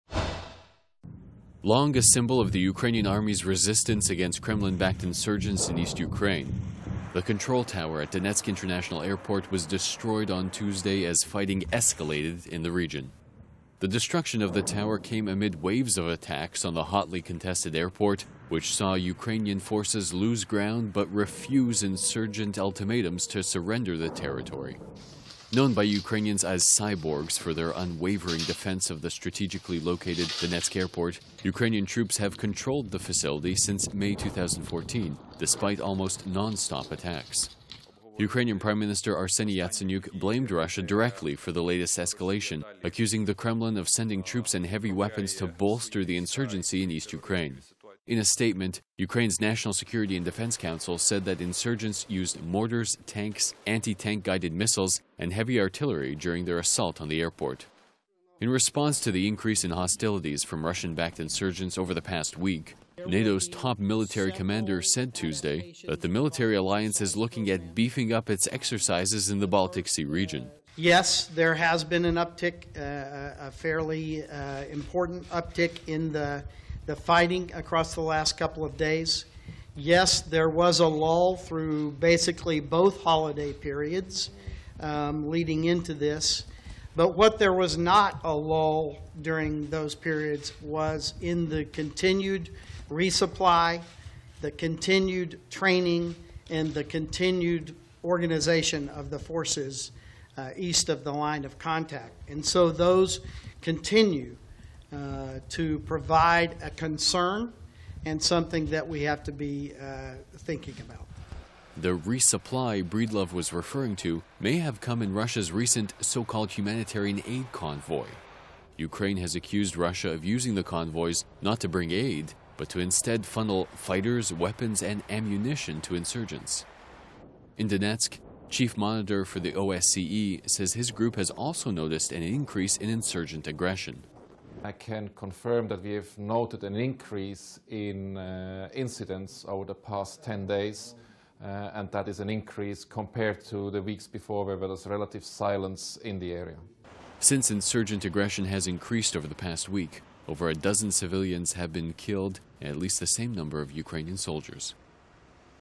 Donetsk Airport Tower Destroyed by Insurgents: Ukrainian 'cyborgs' defending airport since May 2014
Long a symbol of the Ukrainian army's resistance against Kremlin-backed insurgents in eastern Ukraine, the control tower at Donetsk International Airport was destroyed on Tuesday as fighting escalated in the region.